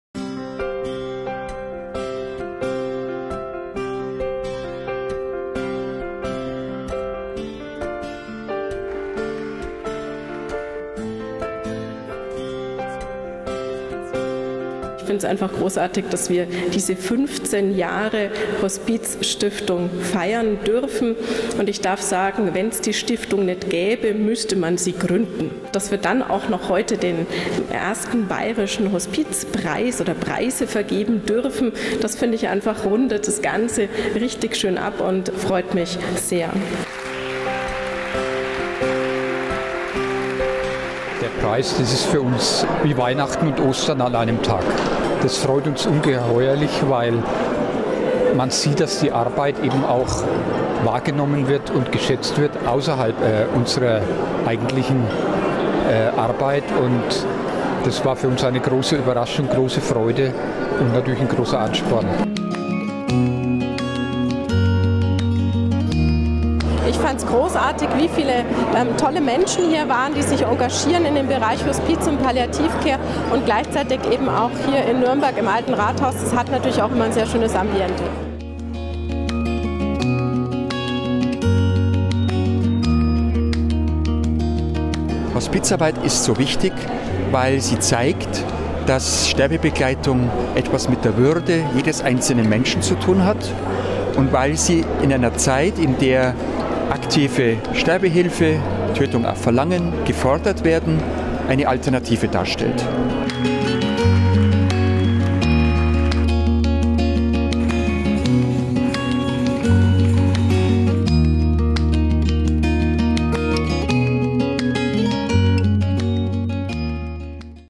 Bayerischer Hospizpreis - Staatsempfang zum 15-jährigen Bestehen der Bayerischen Stiftung Hospiz
Erstmals zeichnete die Bayerische Stiftung Hospiz Ehrenamtliche für ihr Engagement mit den Bayerischen Hospizpreisen aus. Als Schirmherrin der Stiftung überreichte Bayerns Pflegeministerin Melanie Huml am 16. Oktober 2014 die Hospizpreise an Herta Simson aus München und den Hospizdienst DaSein e.V.